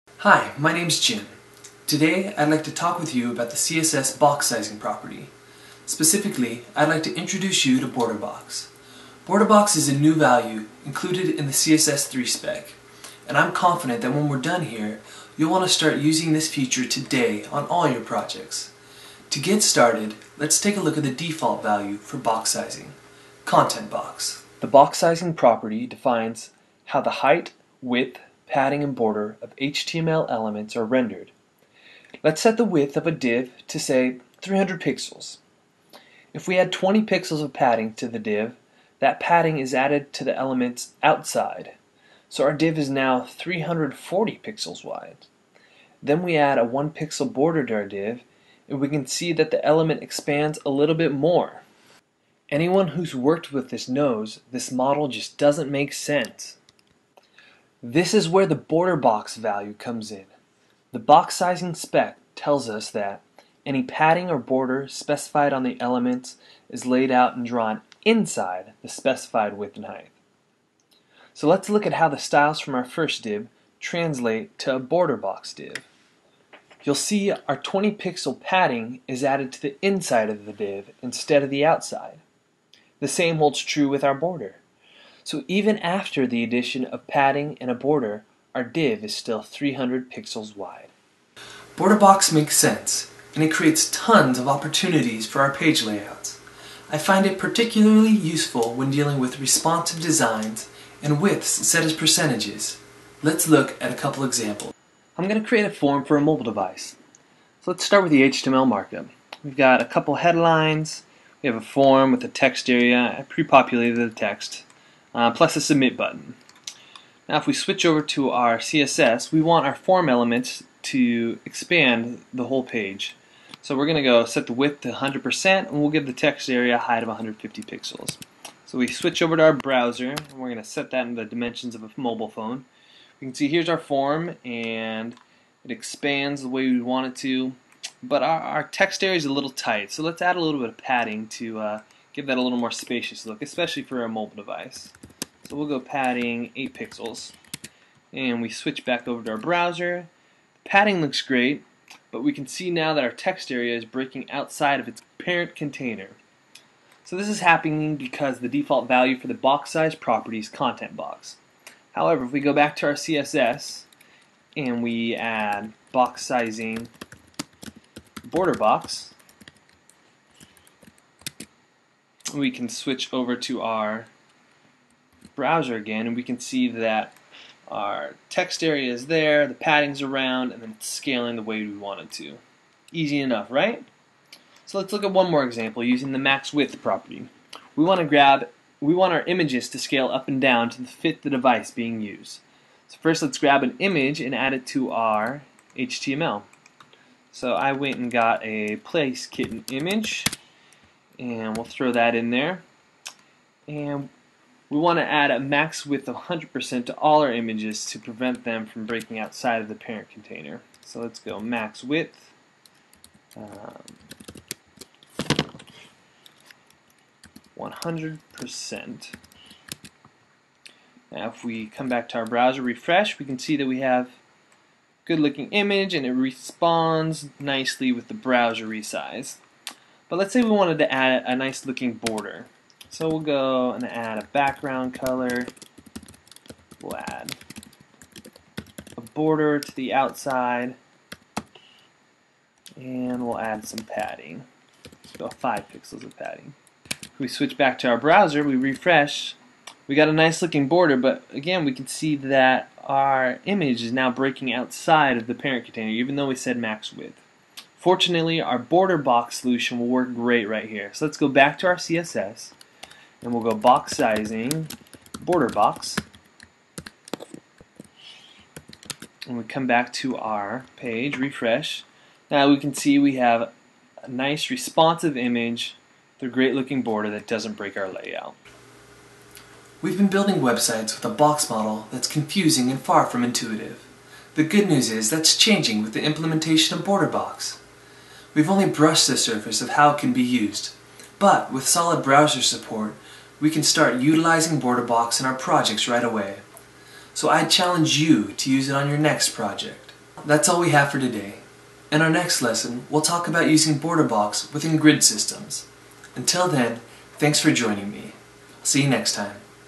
Introduction to the CSS3 border-box
The ol' box-model is not fun, but fortunately the box-sizing property is getting a new value we can use. Learn how and why to start using the new CSS3 value border-box.